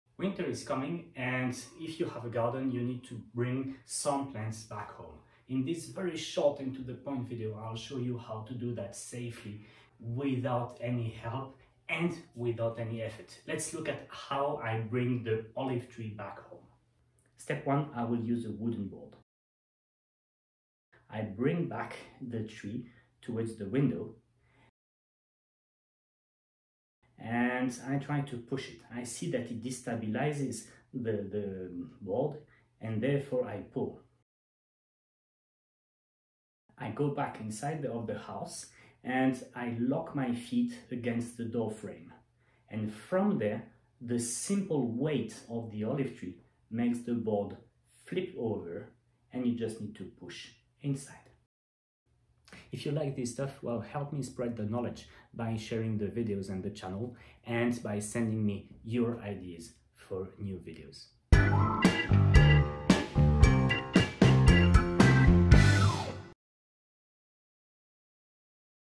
Short And Easy Technique To Move A Tree Effortlessly - Proper Technique = No Back Pain!
Here's a very simple technique to bring your plants back home for the winter. You shouldn't lift above the window frame: this will strain your lower back and can cause acute back pain, lumbago or a disc hernia if your back is fragile. Plants are bloody heavy!

Here, I just use a wooden board and a simple concept: gliding a load is easier than lifting a load. Remember: using tools is easier than correcting your posture. It takes less effort...

Share if you find this useful!